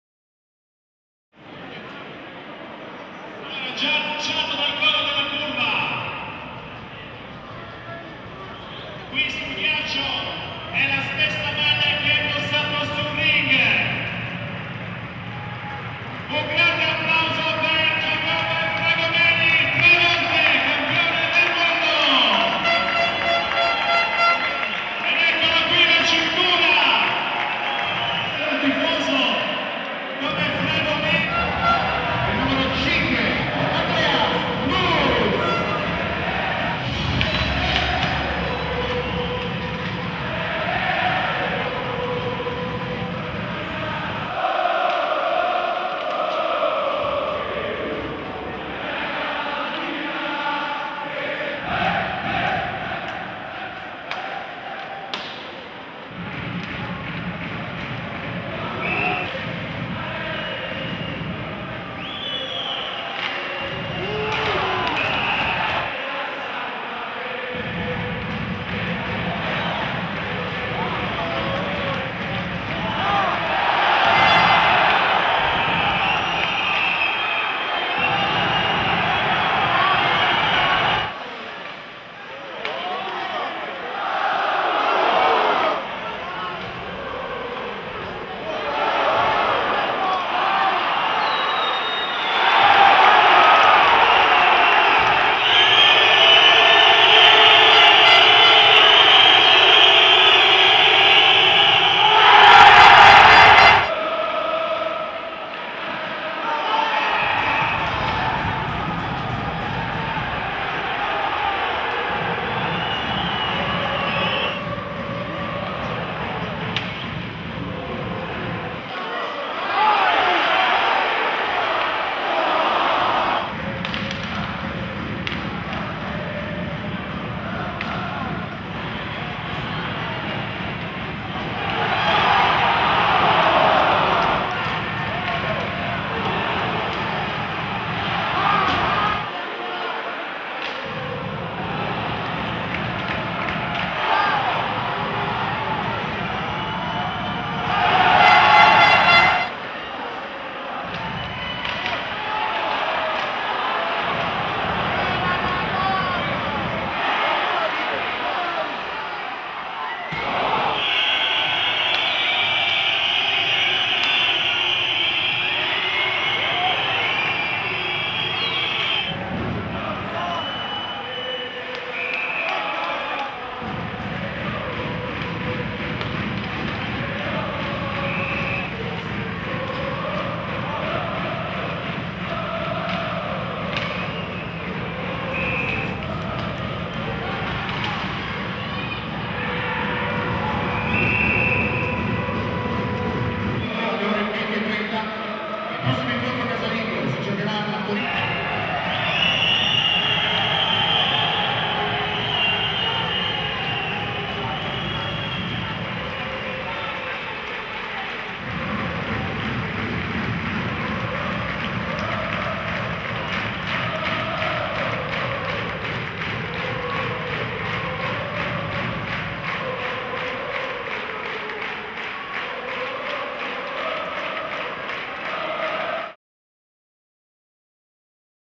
Hockey Milano Rossoblu - H.C. Valpusteria (21 febbraio 2013)
Hockey Milano Rossoblu - H.C. Valpusteria 0-1 (0-1; 0-0; 0-0)
13.15 Pajic (Iannone)